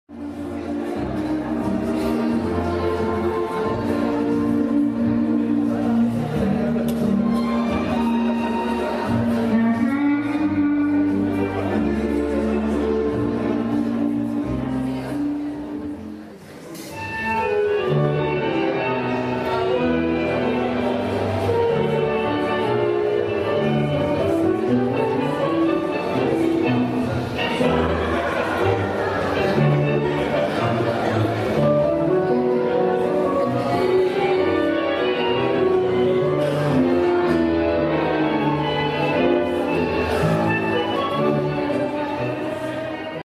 Kevin Costner: Mirada e cabeceo
Kevin Costner & Sean Young in the film "No way out" (in italiano: "Senza via di scampo") year 1987 [Editing: Roberto Finelli]

Ho sostituito l'audio originale del film con una fine cortina + inizio tanda registrate in milonga, per rendere l'idea della mirada e del cabeceo.